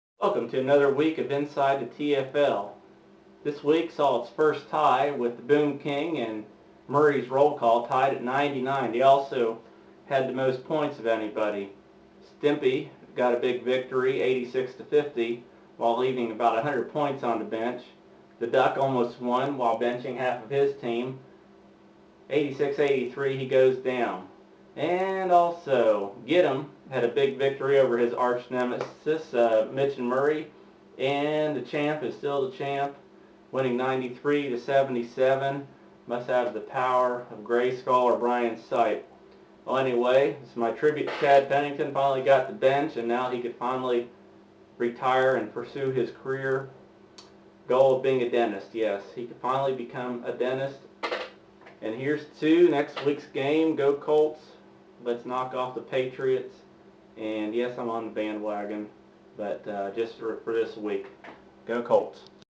TFL Breakdown (30 October)
Pennington benched, Miami still winless, and the TFL has a dogfight for the last playoff spot.  Also, 99-99 tie recorded !